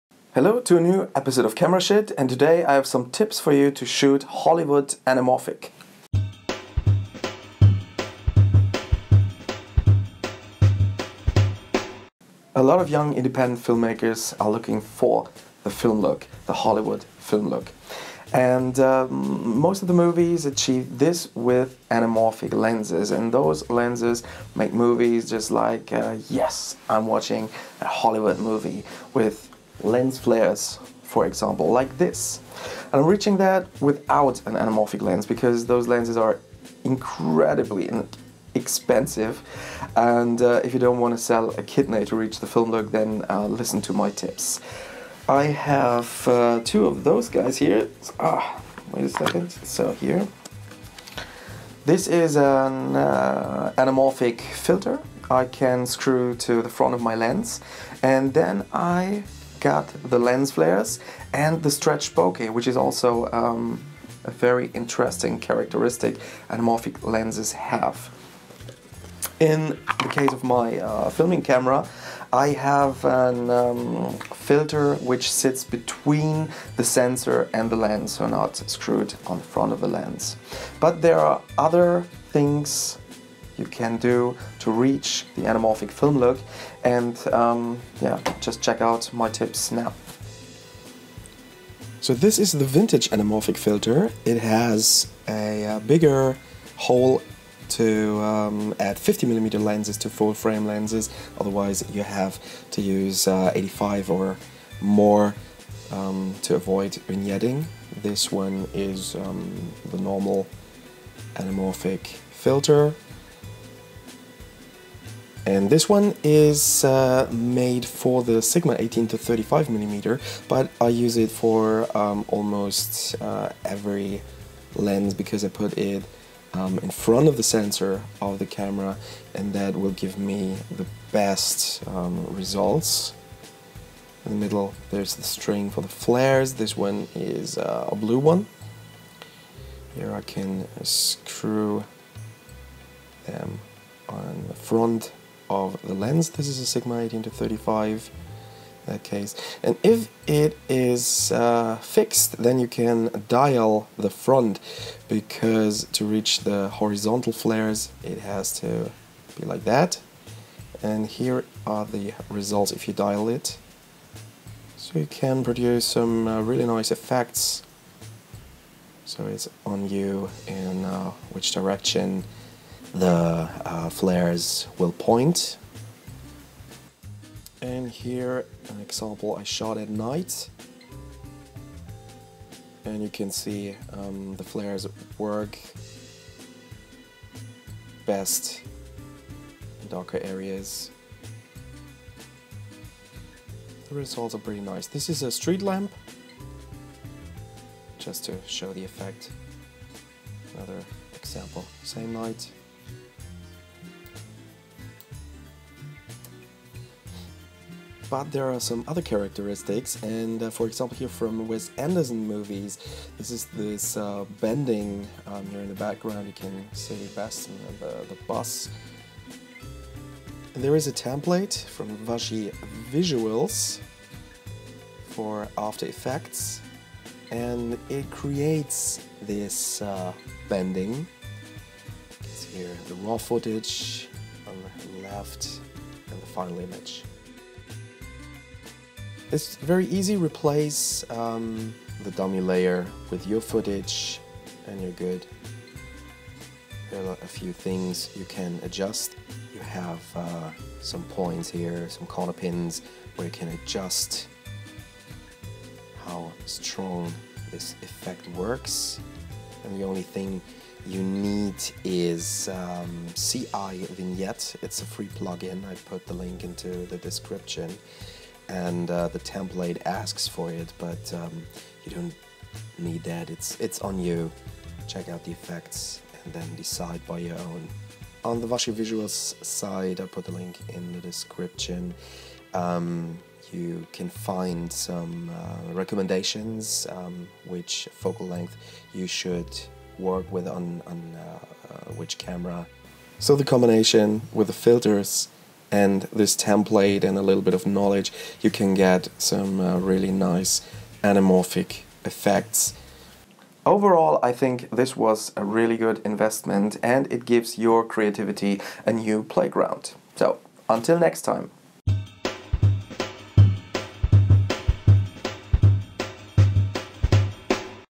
Anamorphic Look Tips
In this episode I share some tips for the anamorphic film look on a budget!